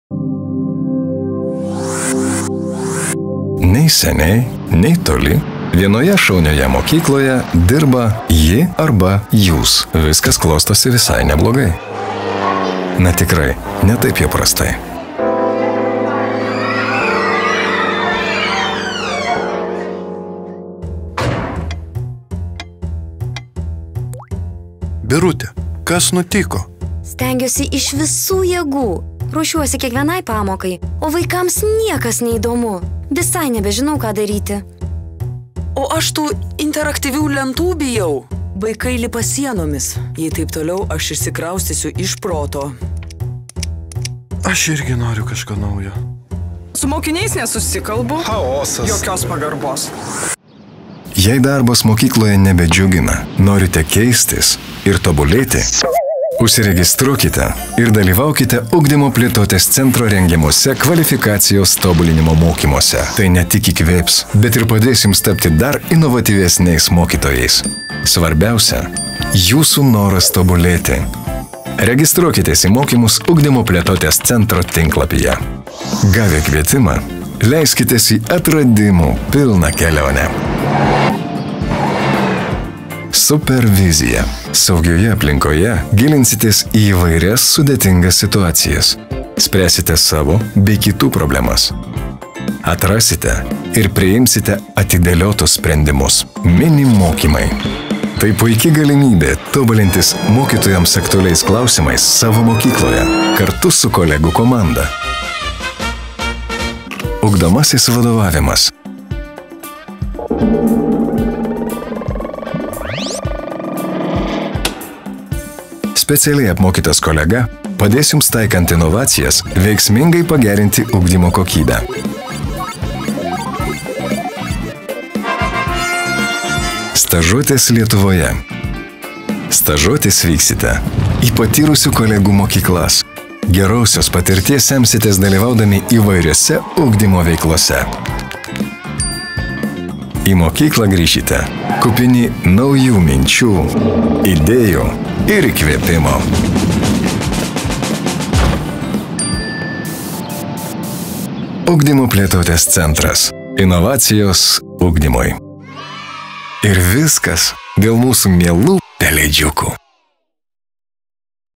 Projekto „Pedagogų kvalifikacijos tobulinimo ir perkvalifikavimo sistemos plėtra“ pristatymas
Pristatomas UPC projektas „Pedagogų kvalifikacijos tobulinimo ir perkvalifikavimo sistemos plėtra (II etapas)“.